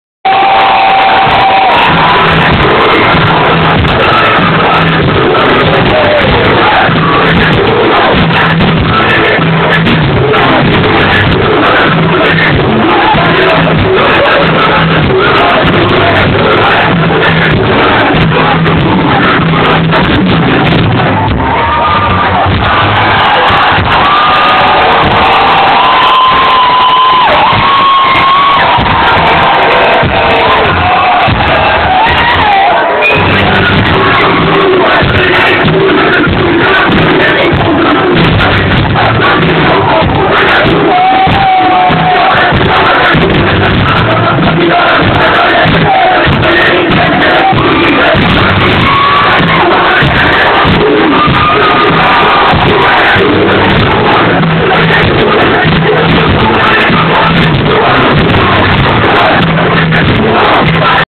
Fatal Bazooka en live !
Voilà les Fatal Bazooka en live au Platinium le 01.09.07